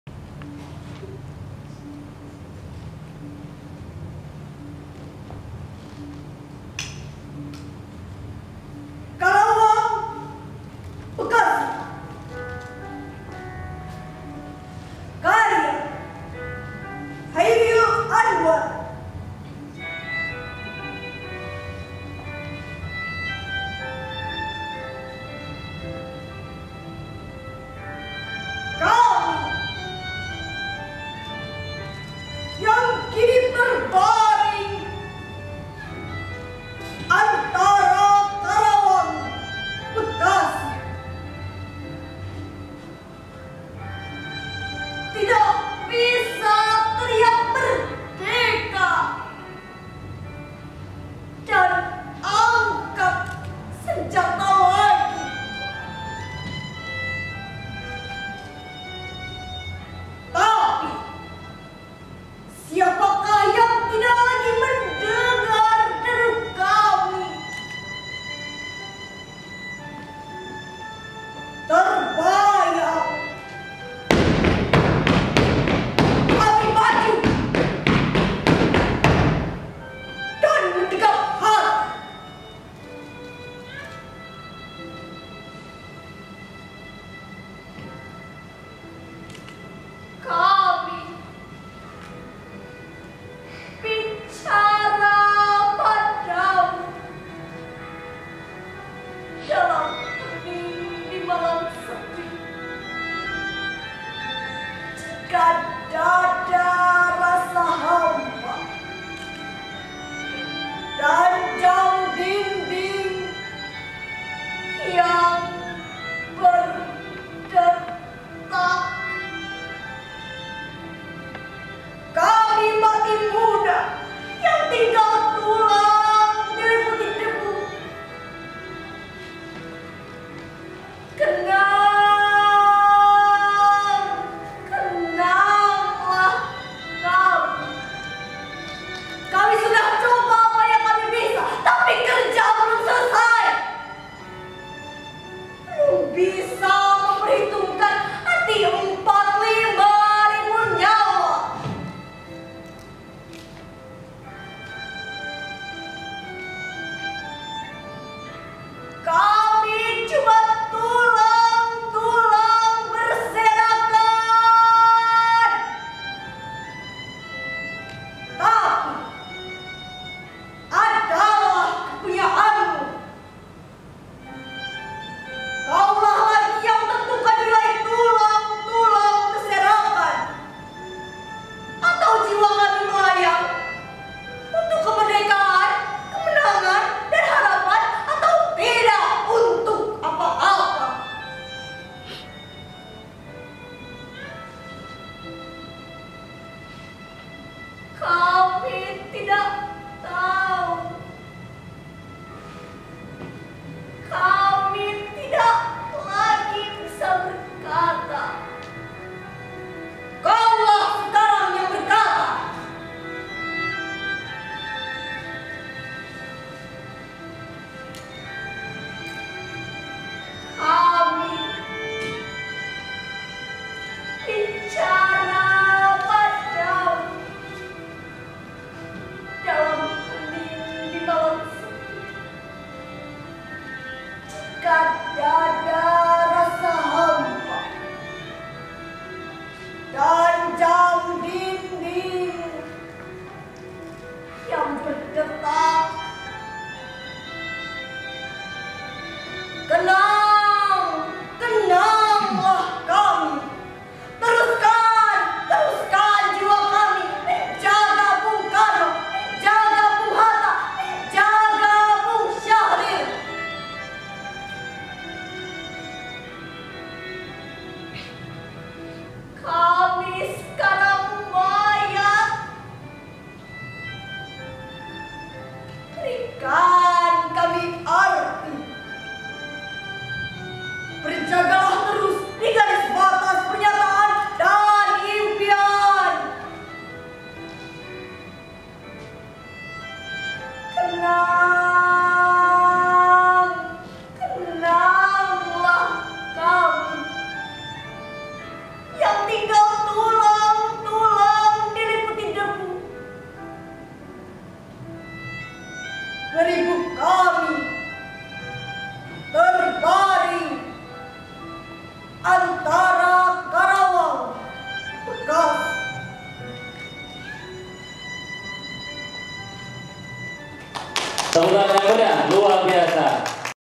Final OSEBI 2020 (Penampilan Puisi SMP) - Clarissa Faza Audrina (Karawang Bekasi)
Clarissa Faza Audrina asal sekolah SMP Negeri 2 Bandung